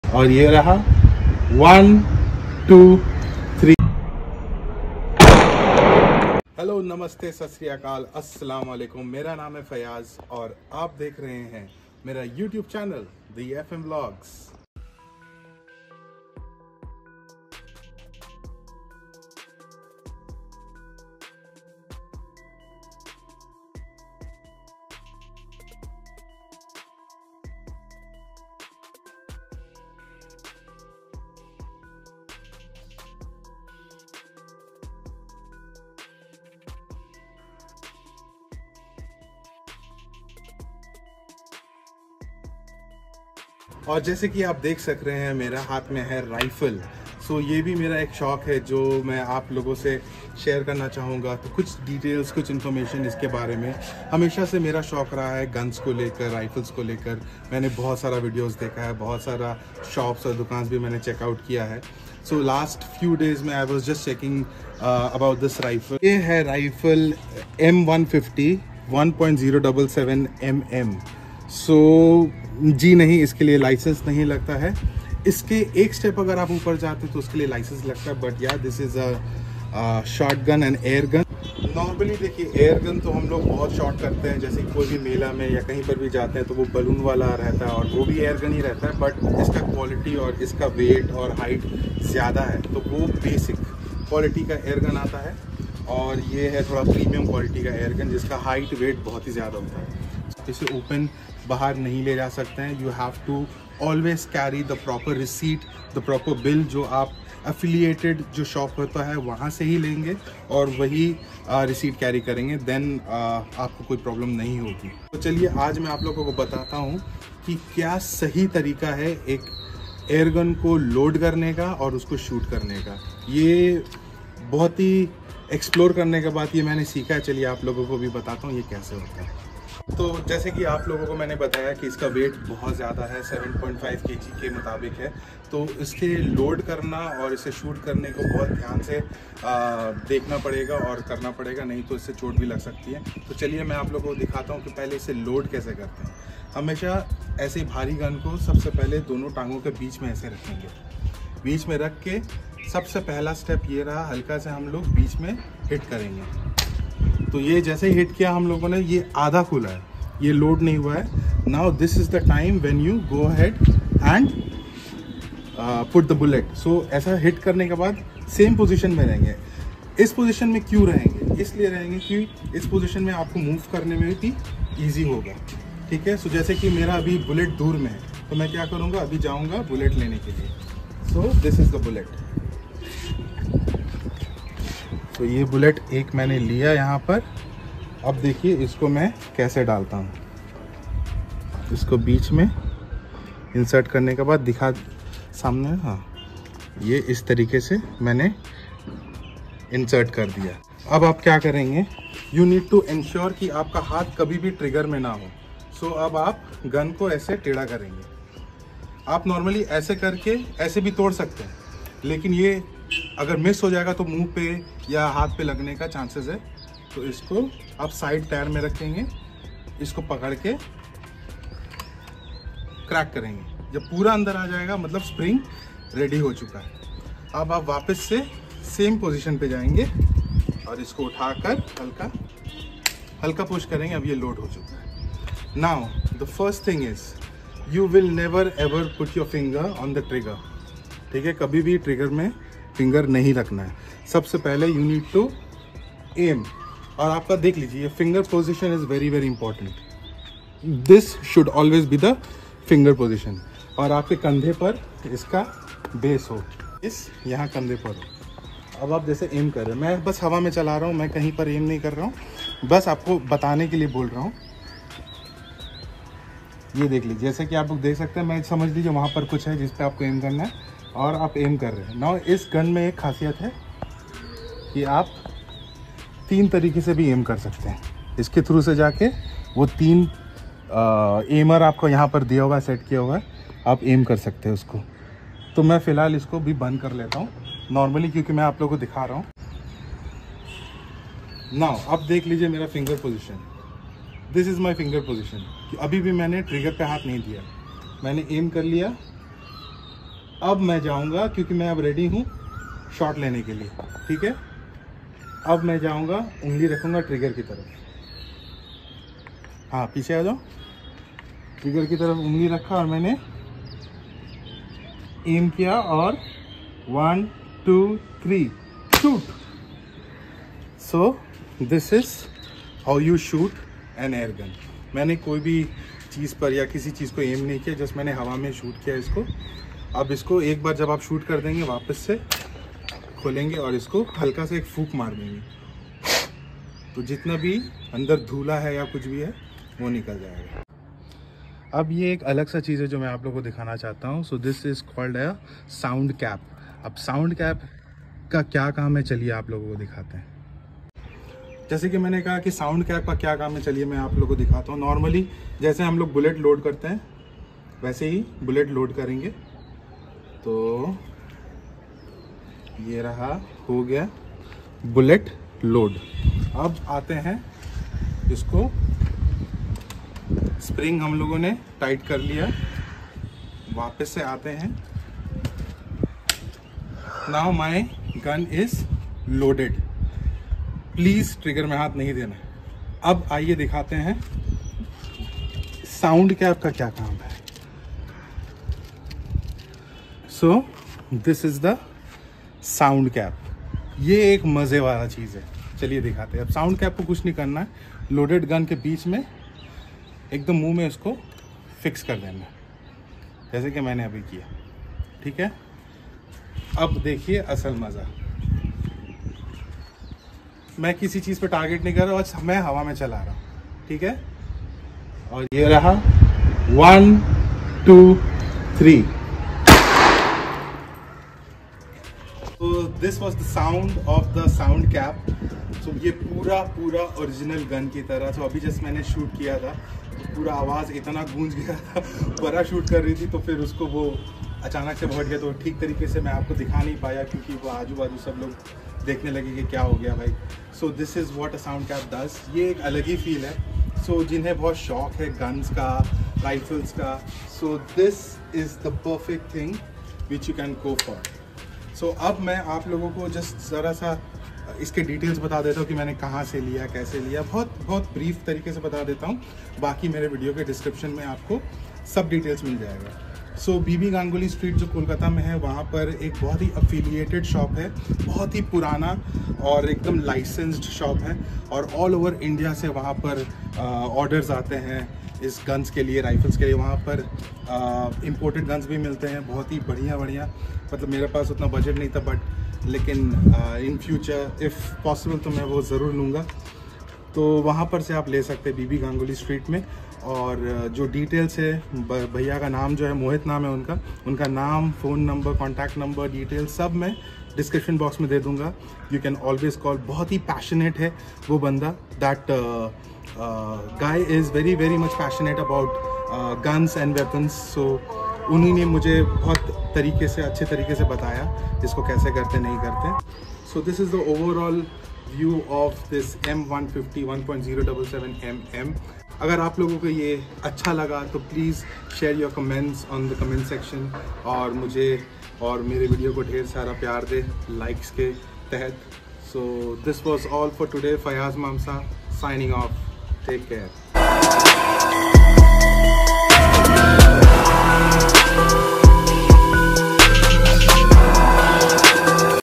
Get Set Shoot | M150 Rifle 1.077 mm | FM VLOGS
This is about the M150 rifle 1.077 mm
Correct way to load and shoot….

Contact - M L Bhunja & Co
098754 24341

Rifle Cost - INR 7500/-